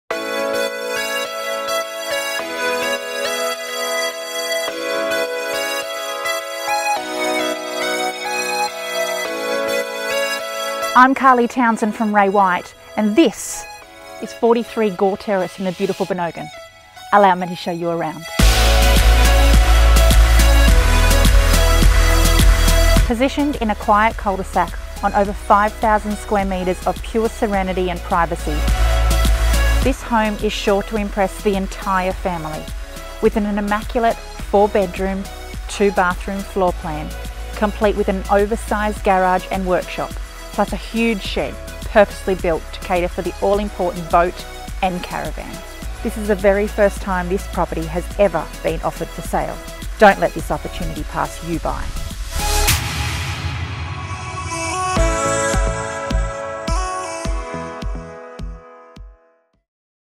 SOLD | 43 Gaw Terrace, Bonogin
The silence is deafening, the privacy is next level and the vehicle accommodation is every mans dream!

Located in a quite cul-de-sac where serenity and privacy is the very essence of your being. This 5022m2 block of land was hand picked by the owners to build their dream home on and they have loved every minute of raising their family here but the time has come for them to downsize as their children leave the nest.

This property is sure to impress, featuring:

* 4 bedrooms all with built in robes and ceiling fans
* Master bedroom complete with walk in robe and en-suite with bath
* Multiple spacious living areas consisting of 2 open plan formal lounge rooms and dining area + the separate children's wing with their own tiled living area.
* Large, timeless timber kitchen with stainless steel appliances including dishwasher and gas cooktop
* Fireplace
* Zoned ducted air conditioning
* High ceilings throughout
* The undercover entertaining deck overlooks the beautiful inground pool
* Oversized double garage with fantastic workshop at the rear
* Massive 10.4 x 9m shed with its own driveway access - ideal to accommodate the caravan, boat and bike - every mans dream!
* Town Water
* 5022m2 block backing onto the summer creek
* Security system

This is the very first time this property has every been offered to the market place, don't let this opportunity pass you buy.